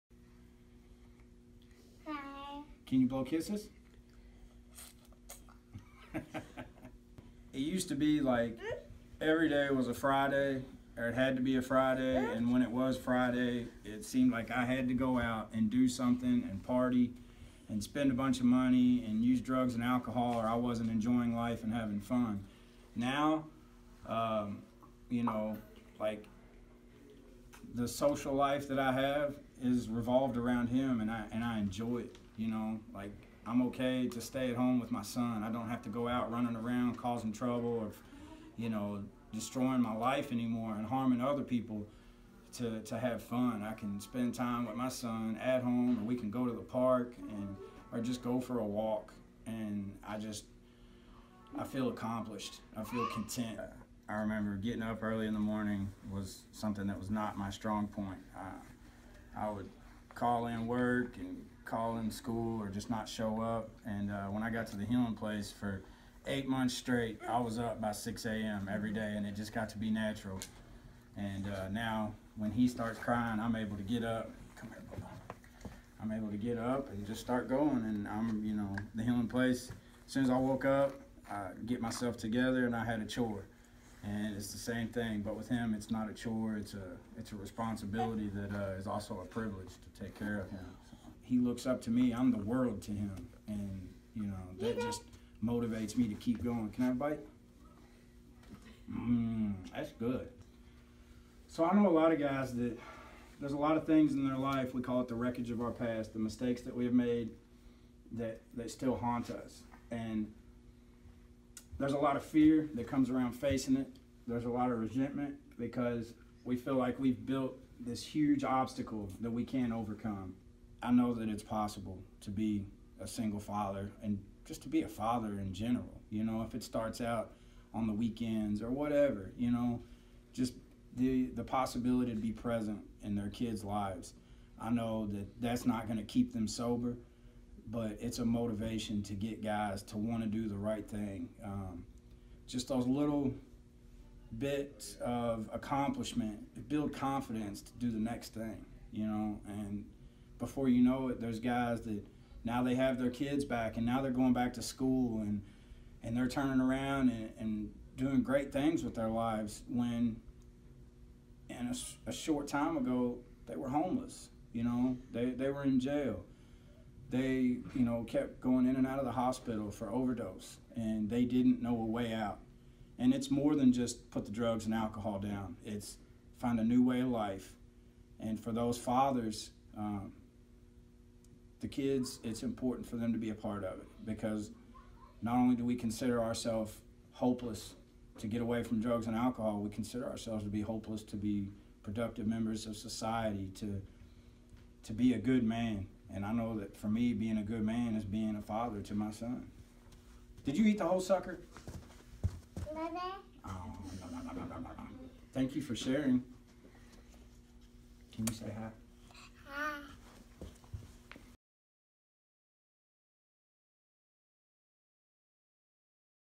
What The Healing Place Does for Clients and Their Children
Not only are the men and women at The Healing Place receiving the tools for recovery and a life free from drugs and alcohol, they are learning how to be better parents for their children – and better children for their parents. They are also starting a new way of life. What happens inside the walls of The Healing Place is nothing short of a miracle. Families are reunited every day.